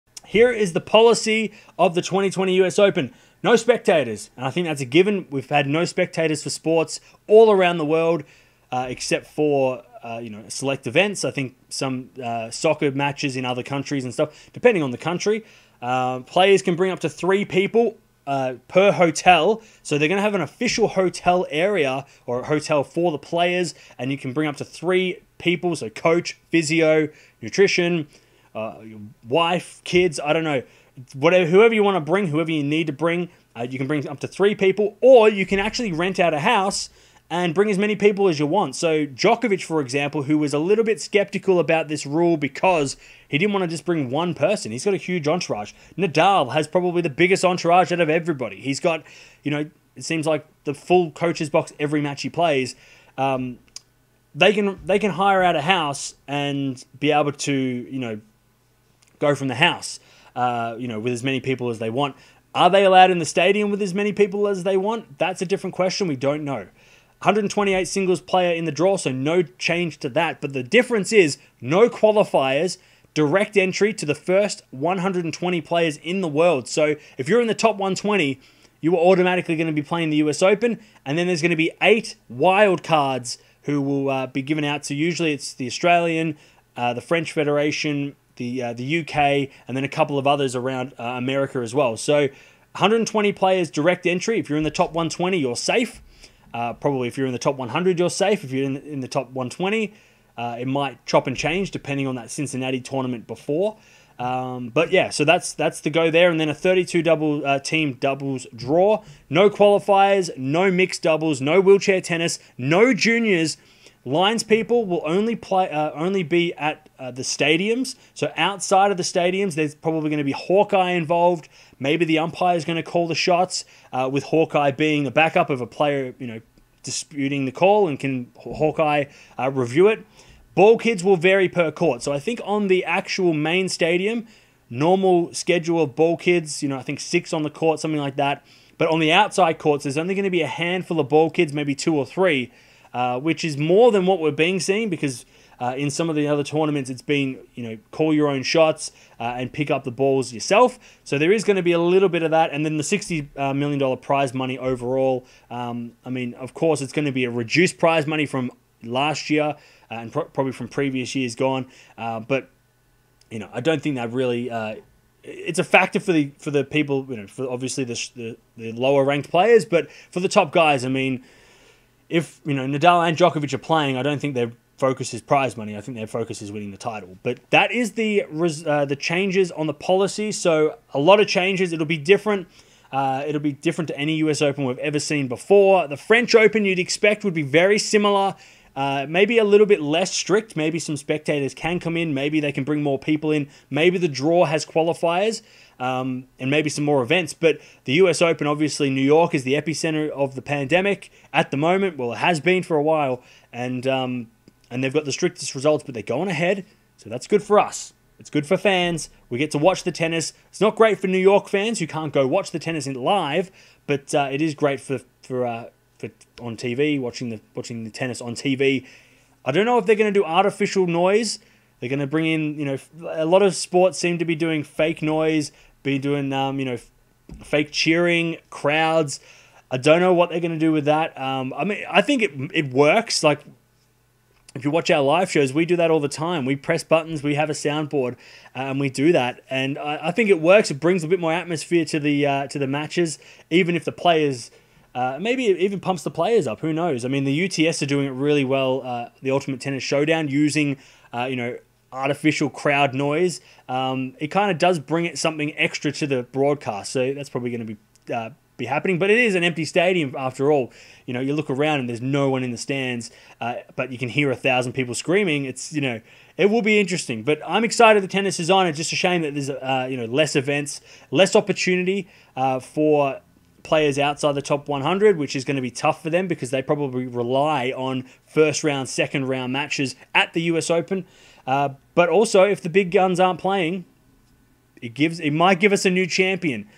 US Open 2020 to Play with Strict Rules | Tennis News
New York Governor Andrew Cuomo gives the green light for the US Open under strict guidelines.